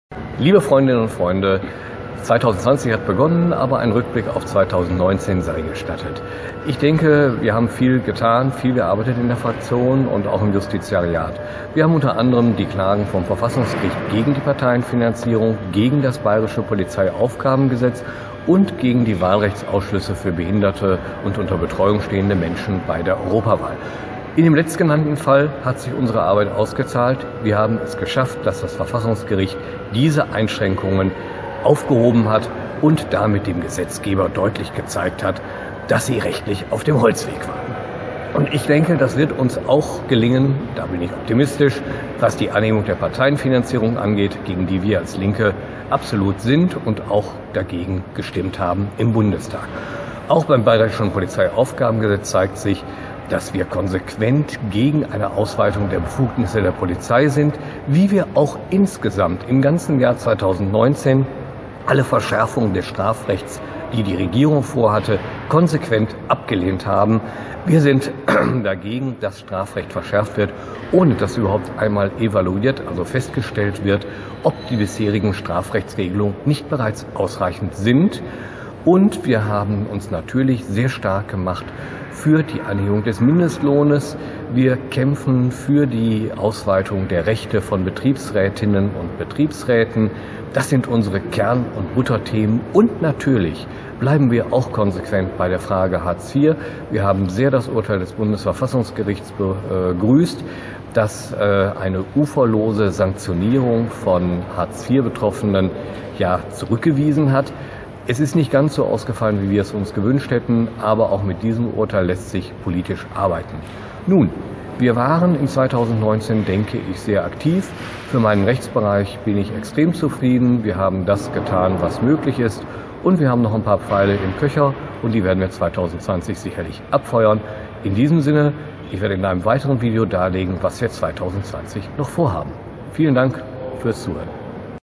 Was wir 2019 erreicht haben
Das neue Jahr 2020 ist schon in vollem Gang. Zeit für einen kurzen Rückblick: Wofür haben sich die Fraktion DIE LINKE im Bundestag und ich 2019 eingesetzt, und was haben wir erreicht?